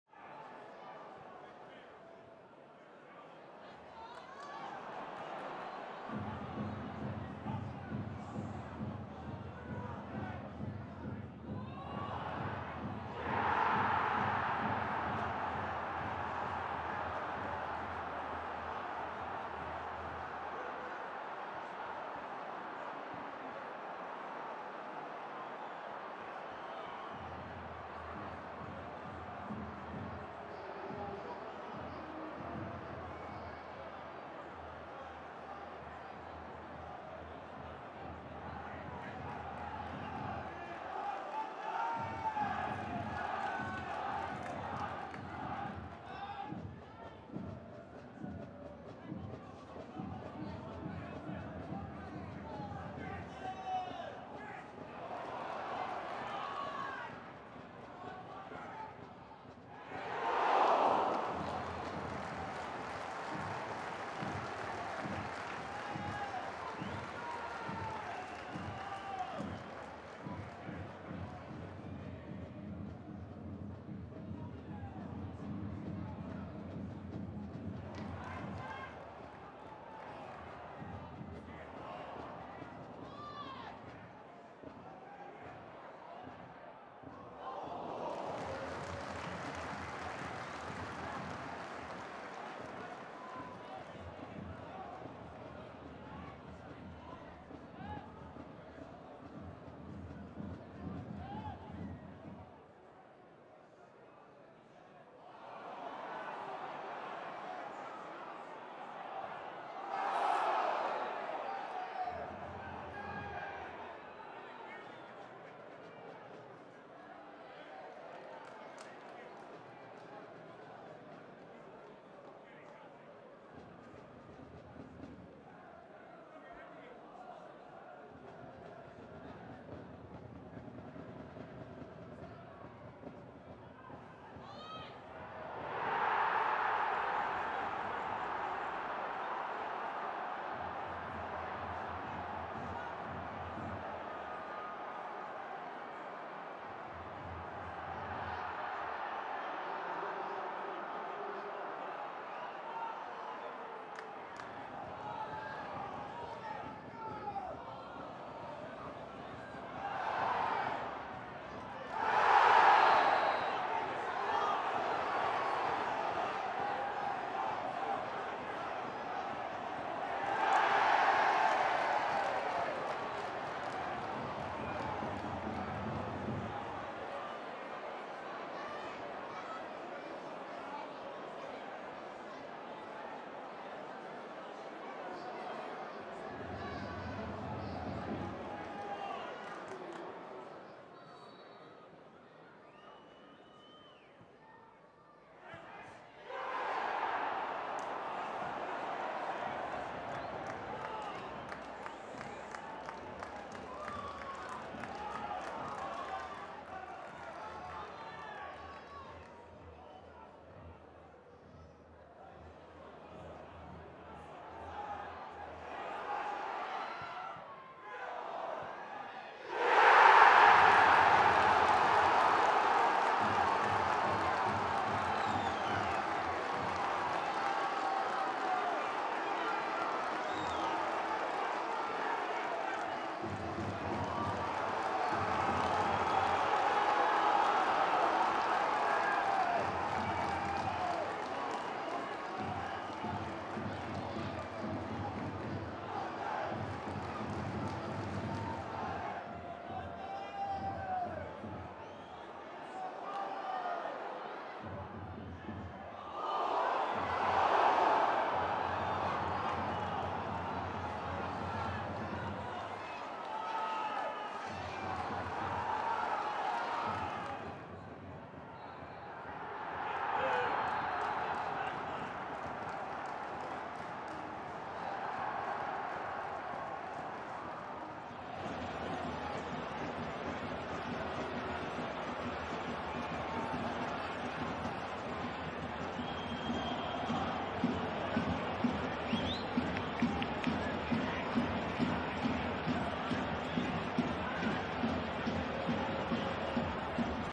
Dundee 1-2 Hibernian | Ten-Man Hibs Hold On To Take Victory! | cinch Premiership
Hibernian hold onto a 2-1 lead despite Lewis Miller being sent off in the second half.